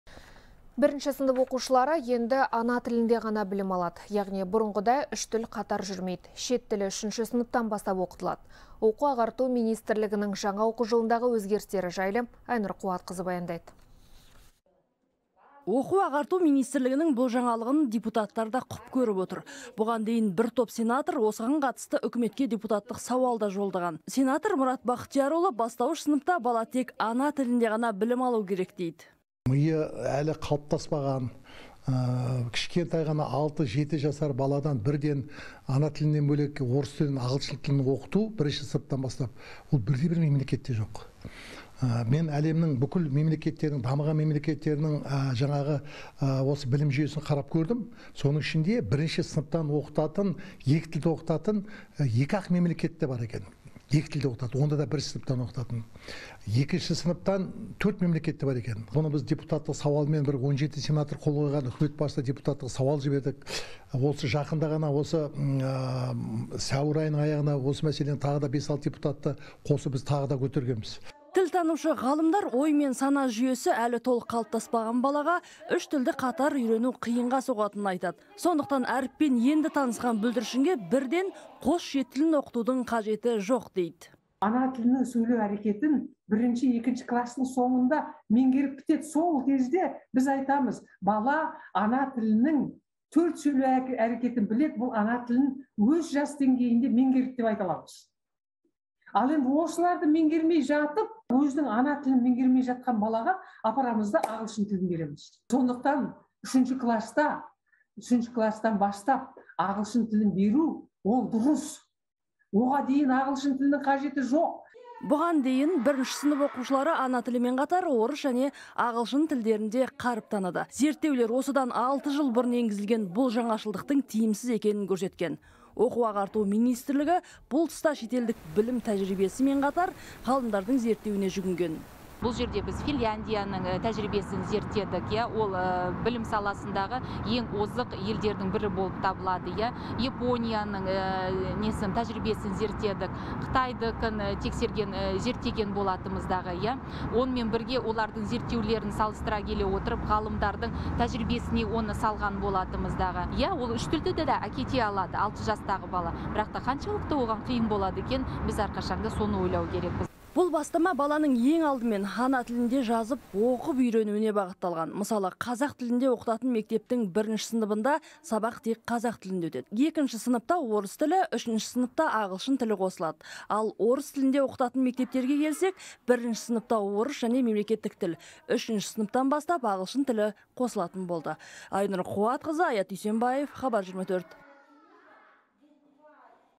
1-сынып оқушылары тек ана тілінде білім алады - Аймағамбетов
Оқу-ағарту министрлігінің бұл жаңалығын депутаттар да құп көріп отыр. Бұған дейін бір топ сенатор осыған қатысты үкіметке депутаттық сауал да жолдаған, деп хабарлайды «Хабар 24».
Сенатор Мұрат Бахтиярұлы, бастауыш сыныпта бала тек ана тілінде ғана білім алуы керек, дейді.